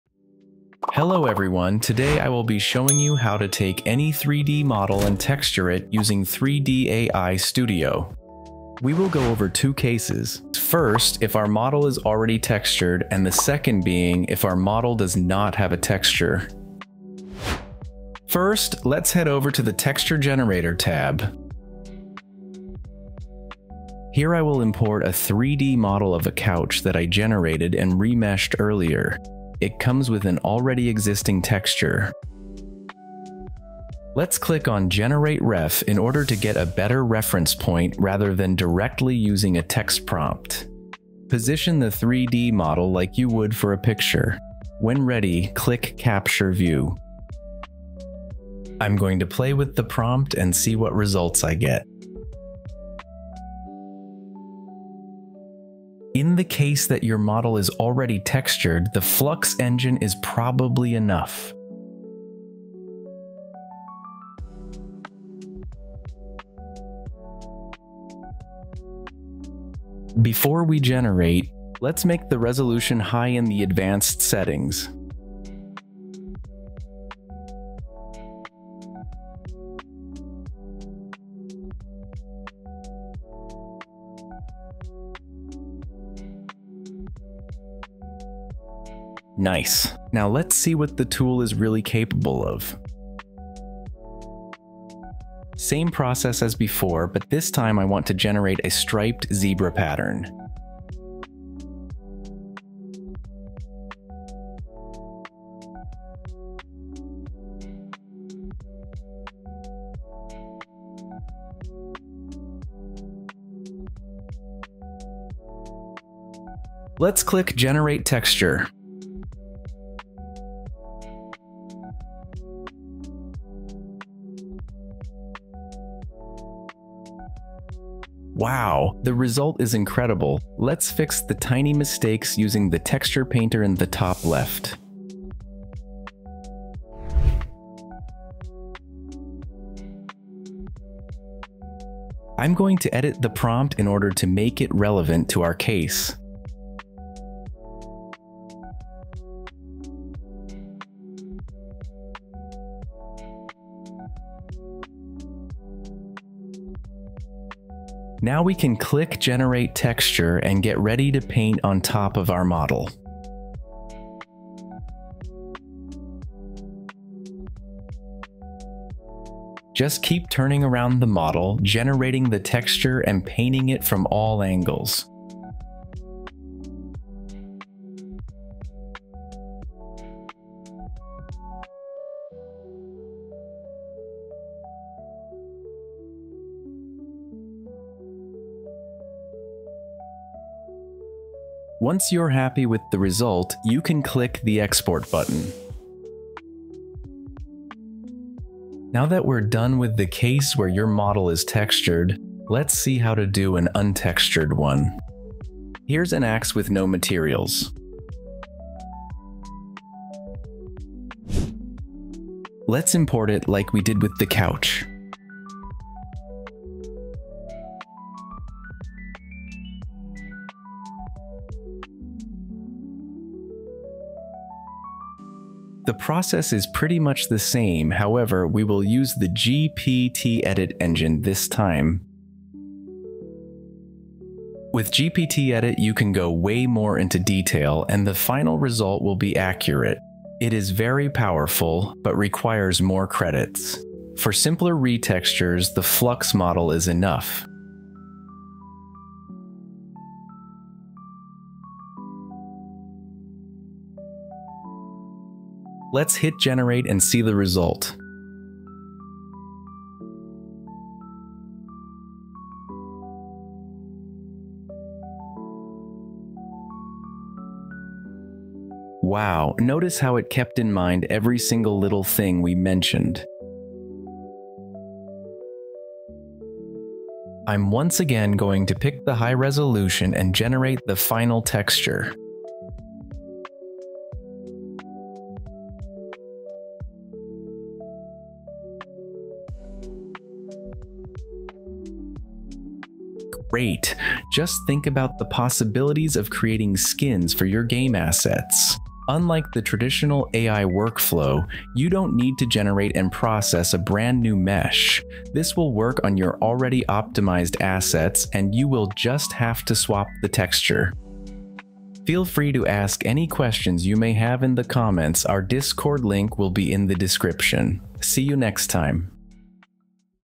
How to Texture Any 3D Model Using AI
Discover the amazing texture AI tools that 3daistudio has to offer!

📽️In this video we will show you the Texture Generator tool and how to use it to texture your 3D models. 🖌️

We will go over two cases.
1. Your 3d model already has a texture.
2. Your 3D model has no textures.

The latest update brought to the Texture generator brings a reference generation tool, which is what we will be taking advantage of in this video, in order to generate texture references that match our imagination.
No more guesswork using text for the prompt!